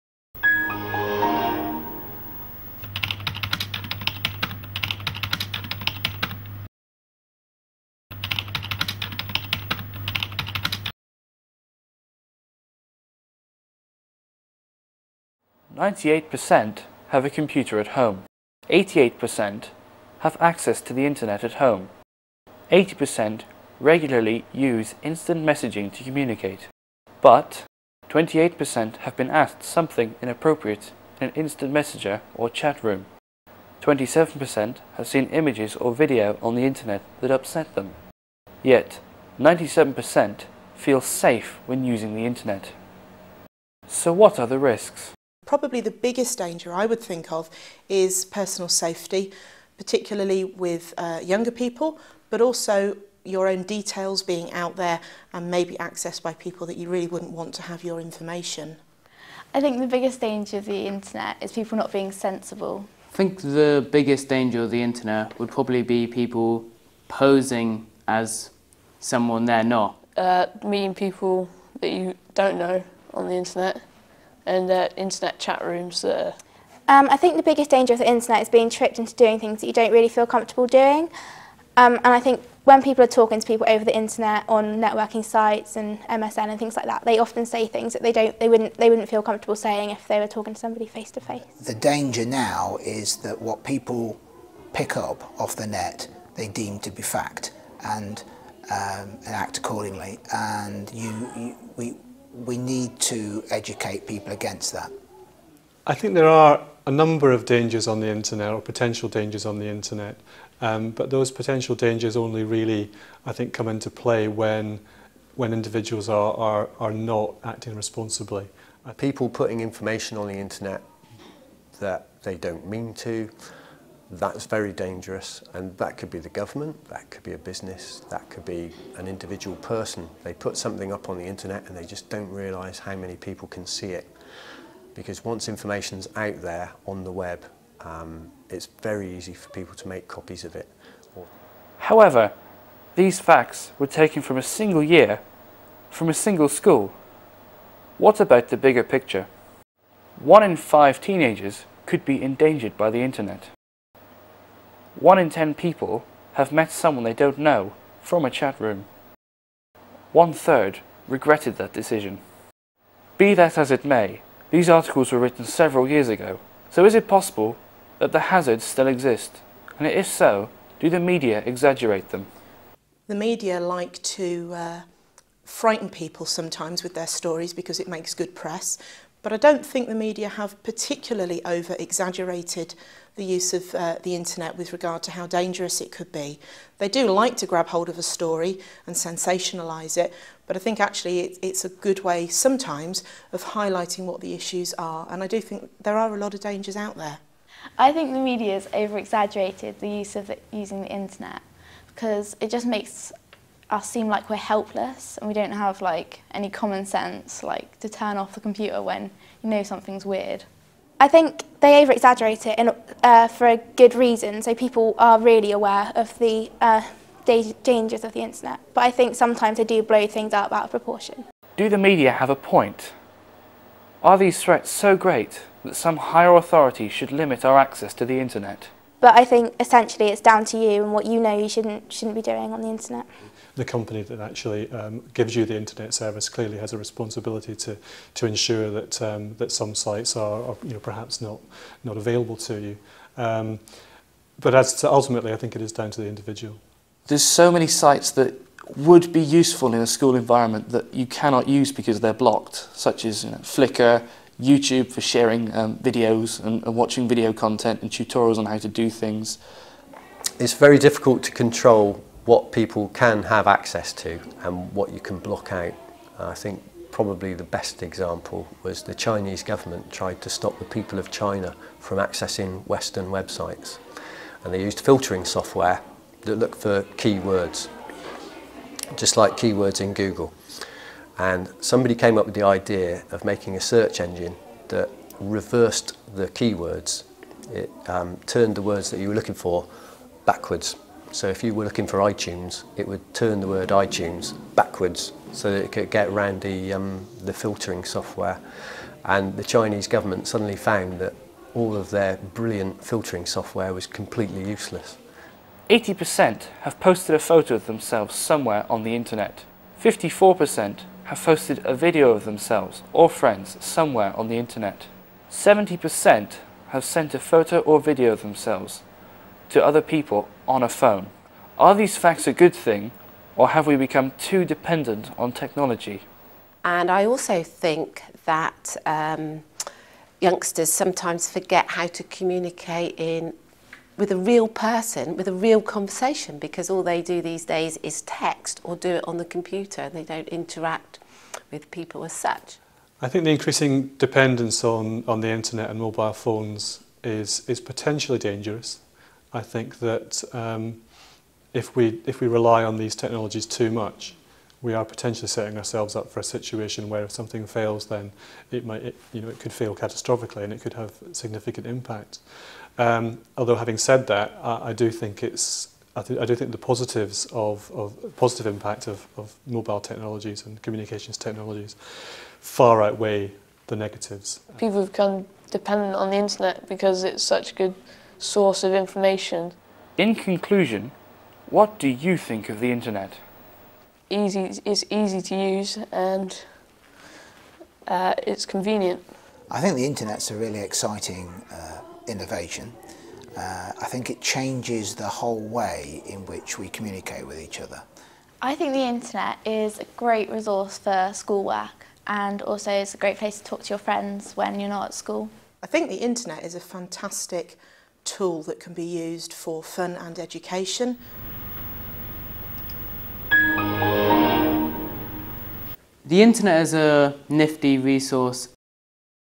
Stay Safe
Year 11 students at Tiverton High School worked with Devon Educational Services to create a short film about Internet Safety.